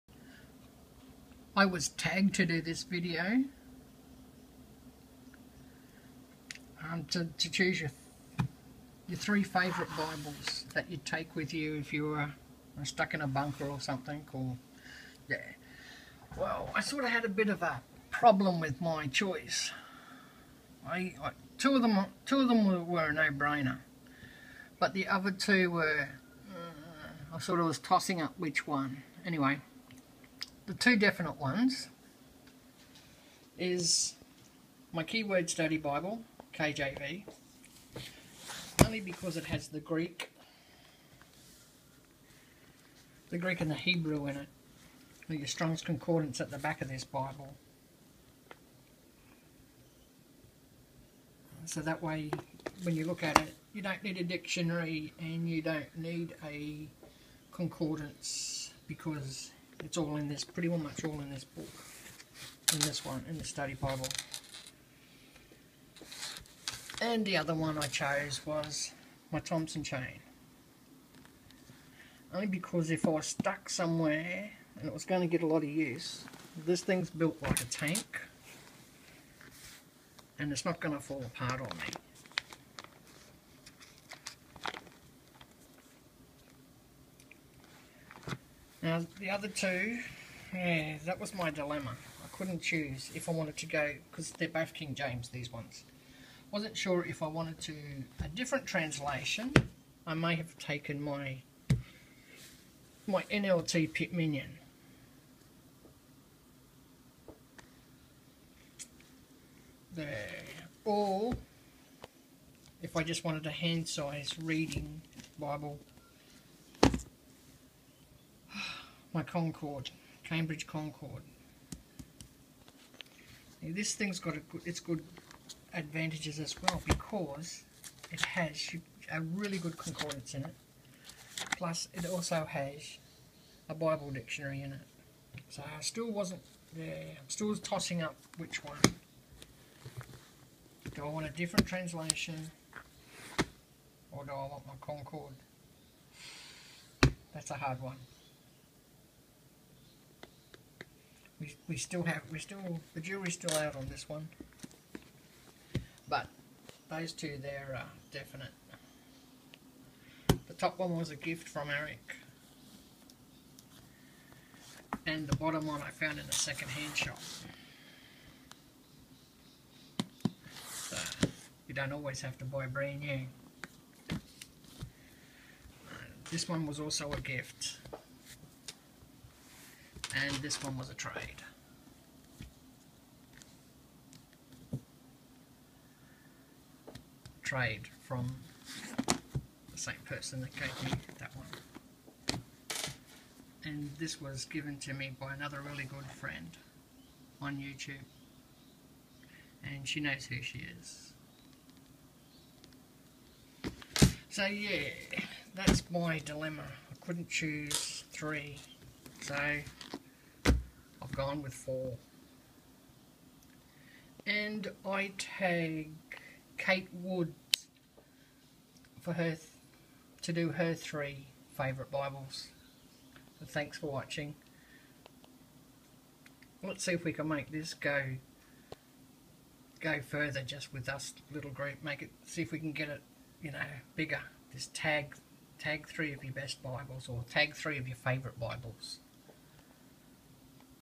Tag you're it challenge
I was tagged by Airik1111 to post 3 bibles I would take with me if I was in a bunker etc, and I tag Cat Woods